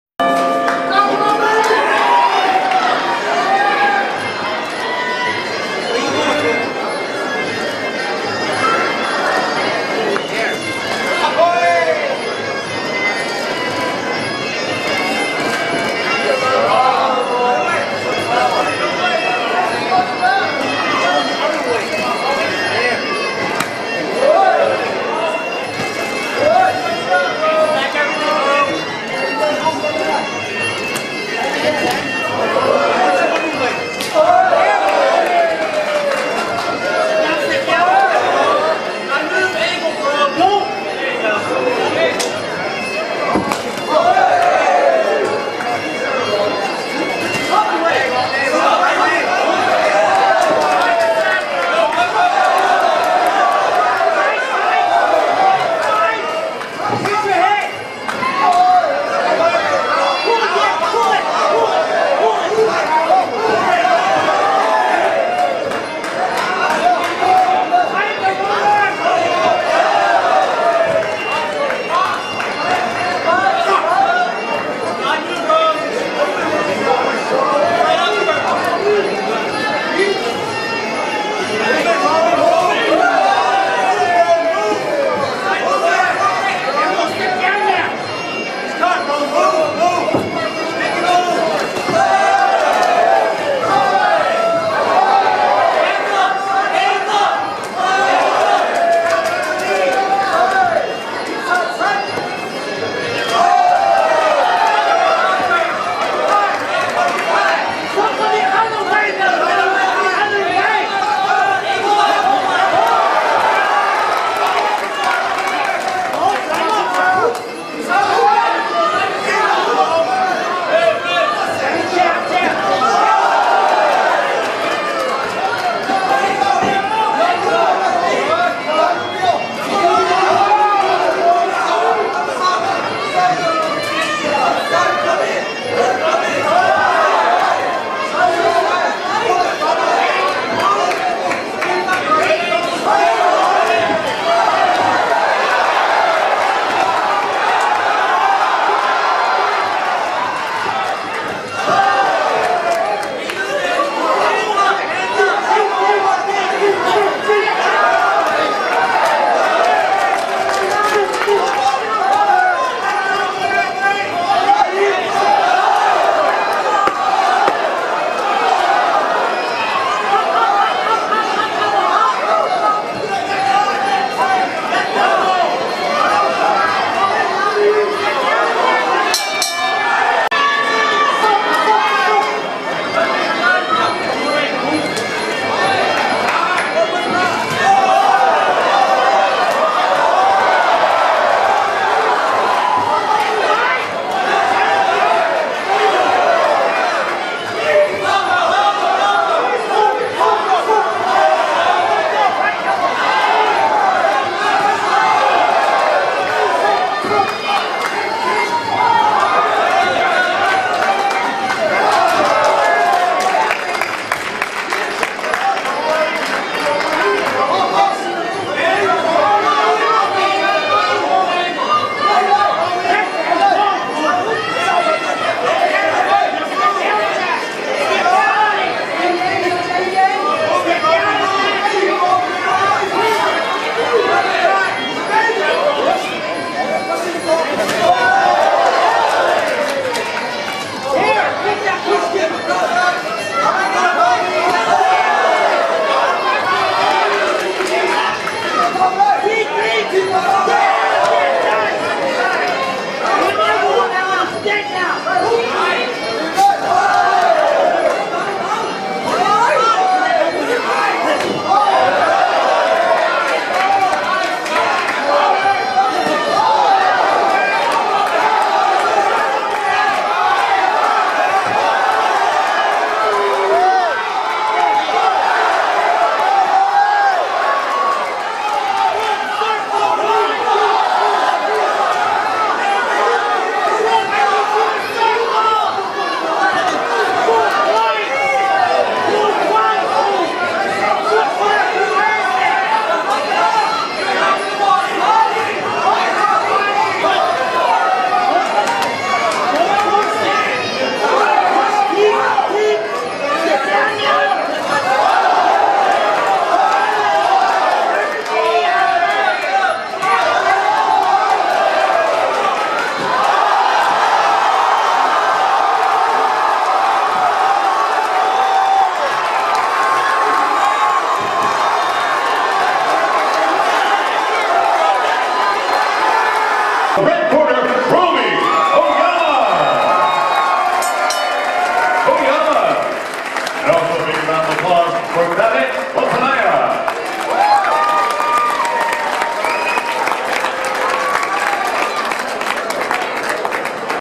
Romie Adanza Vs Boom Wathanaya 3rd Fight
Rubber match between Romie Adanza and Thannit "Boom" Wathanaya.
@ Hollywood Park Casino , WCK Cali Three Event